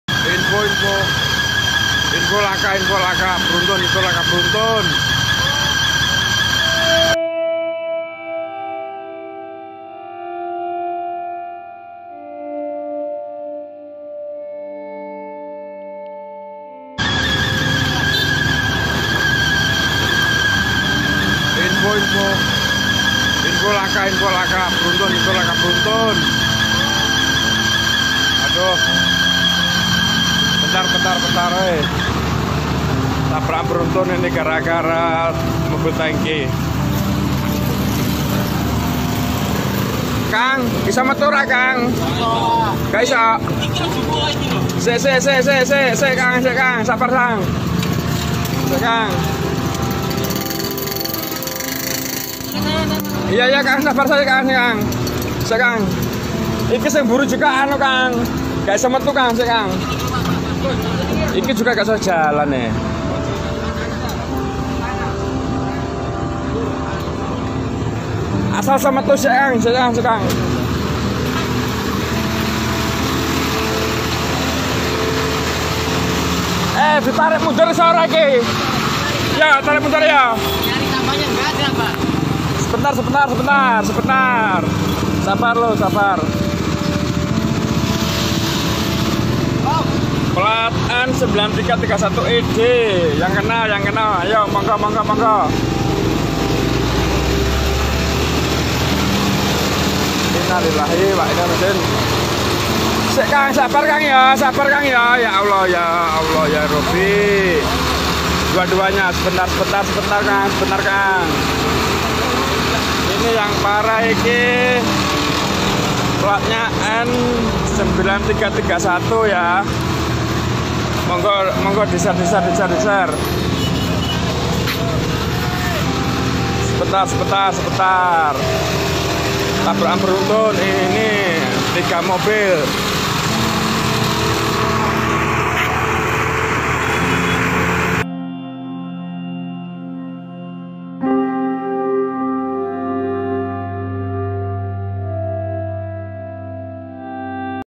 L4K4 BERUNTUN || LOSARANG, INDRAMAYU
Kecelakaan beruntun di Losarang, Indramayu Jawa barat, Minggu pagi 05/06/2022.